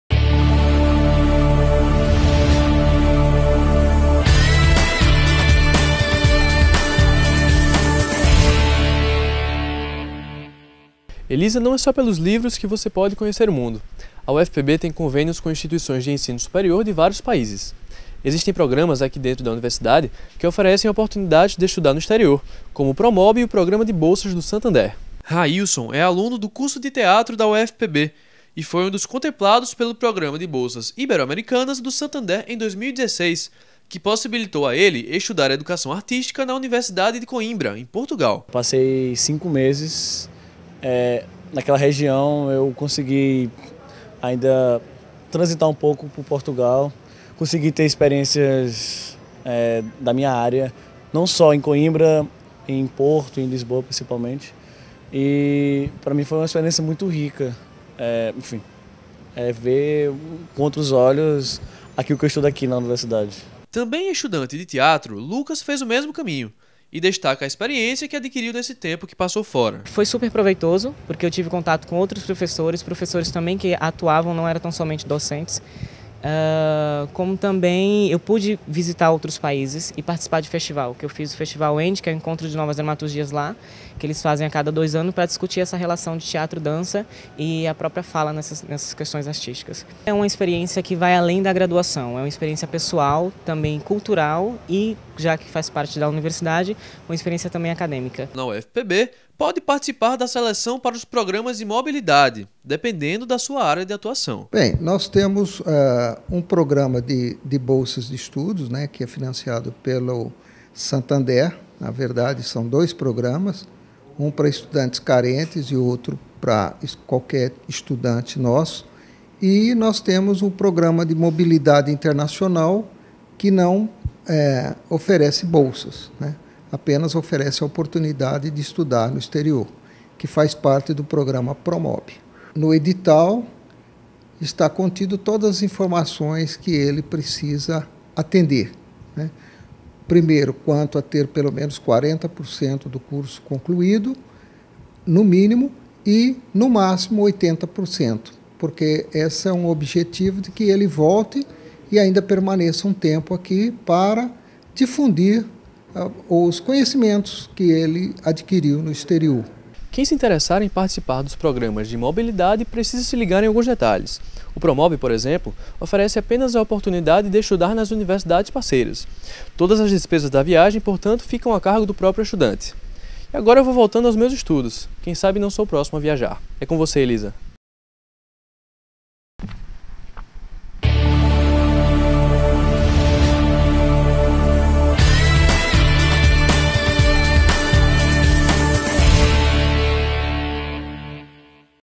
UFPB Acontece - INTERCAMBIO
A UFPB TEM CONVÊNIO COM INSTITUIÇÕES UNIVERSITÁRIAS DE VÁRIOS PAÍSES. EXISTEM PROGRAMAS, AQUI DENTRO DA UNIVERSIDADE, QUE OFERECEM OPORTUNIDADES PARA ESTUDAR NO EXTERIOR, COMO O PROMOBI E O PROGRAMA DE BOLSAS DO SANTANDER.
QUALQUER ALUNO DE GRADUAÇÃO MATRICULADO NA UFPB PODE PARTICIPAR DA SELEÇÃO PARA OS PROGRAMAS DE MOBILIDADE, DEPENDENDO DA SUA ÁREA DE ATUAÇÃO.